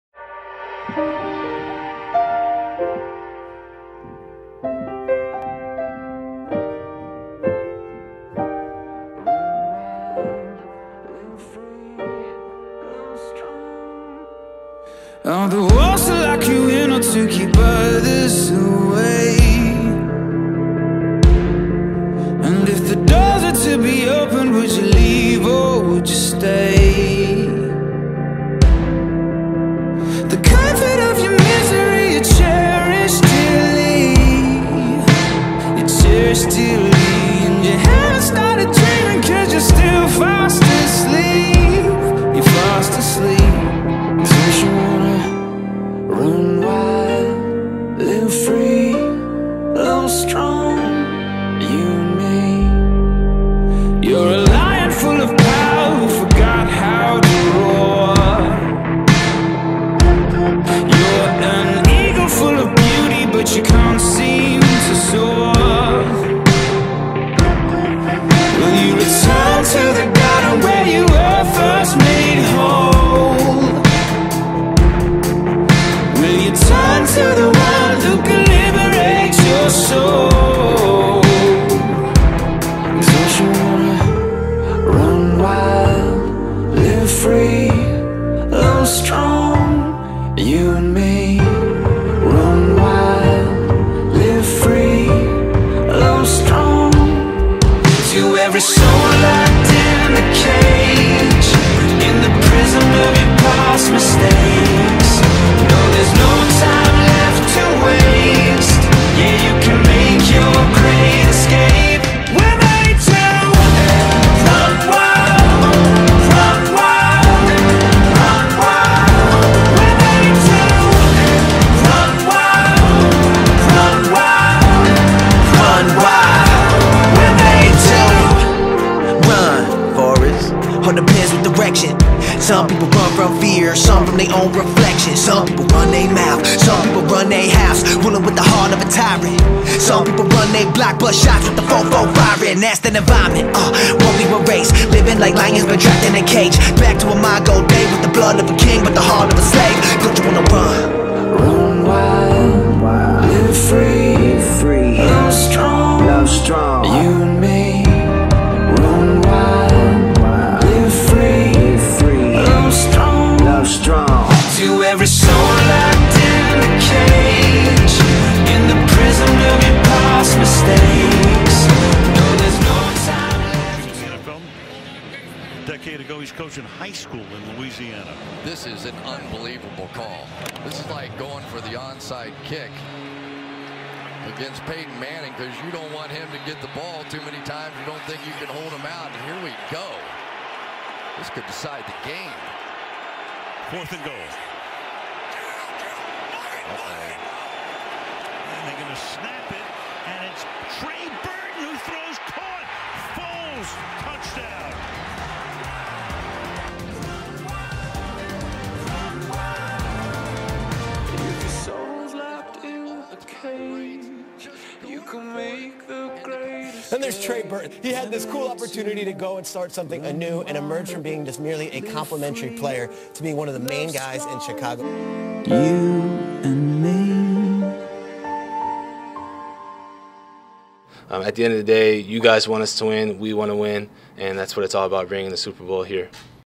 Trey Burton Highlights
QB in high school, WR at the University of Florida, and Undrafted TE in the NFL, A great role model and friend to me and my fam.